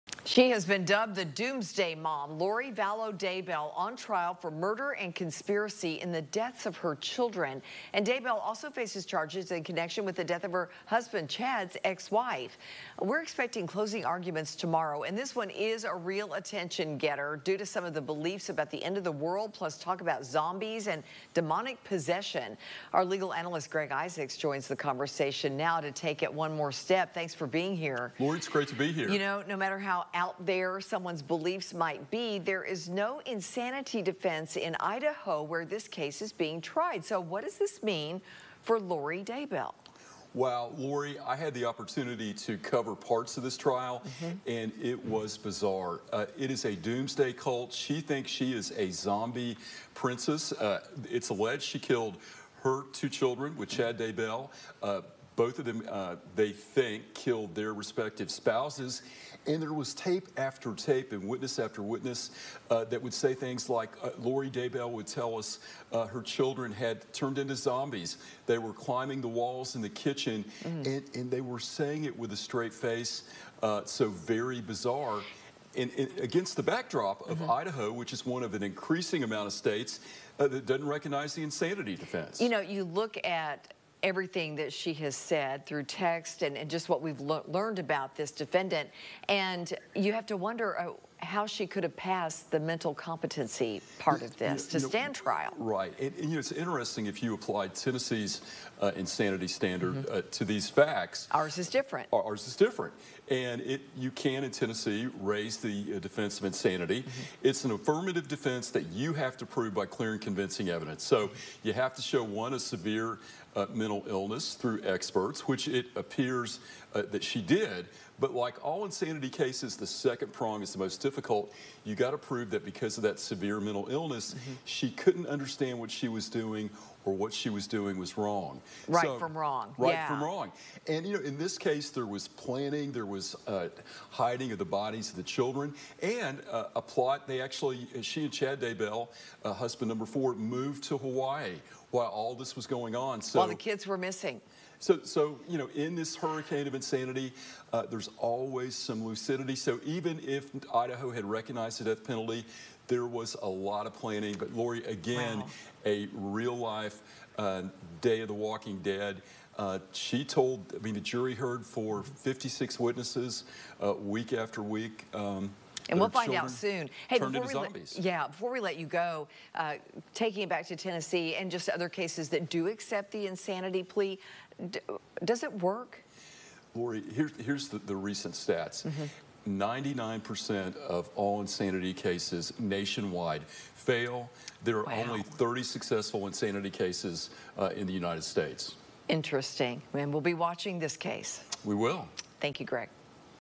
Does the insanity defense work? Doomsday mom trial underway
Prosecutors say Lori Vallow Daybell, the mother charged in a triple murder case, took part in a complex conspiracy that included unusual spiritual beliefs and efforts to kill those who stood in the way of her goals. WATE's Lori Tucker and Lawyer Analyst Greg Isaacs discuss the trial and Idaho's lack of insanity defense.